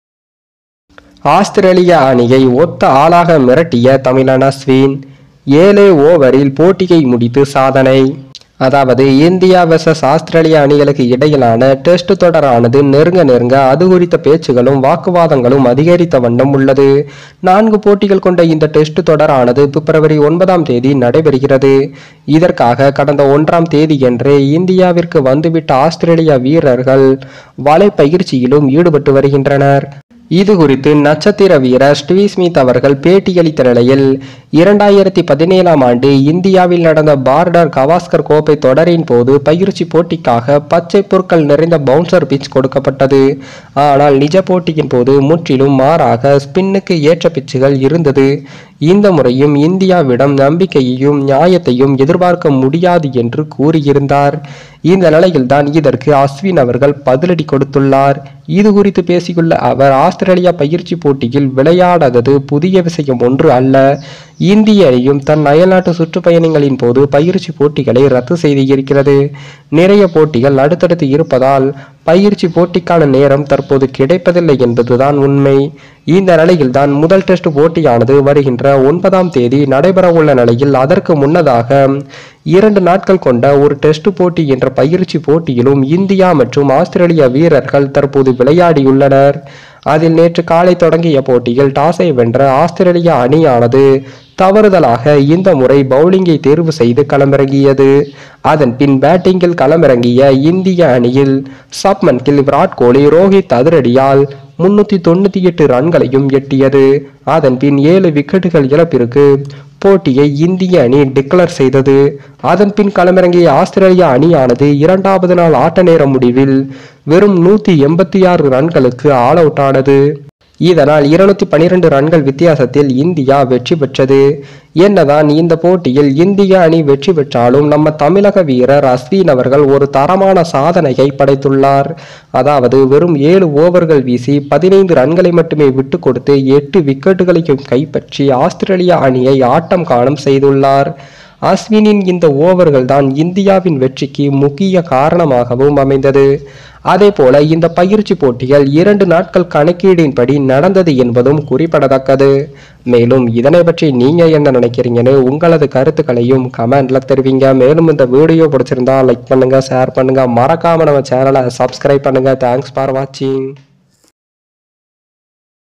IND vs AUS Test: ஏழு ஓவரில் போட்டியை முடித்த அஸ்வின்! மண்டியிட்ட ஆஸ் அணி! உலக சாதனை பேட்டிங்!
support frd's 🙏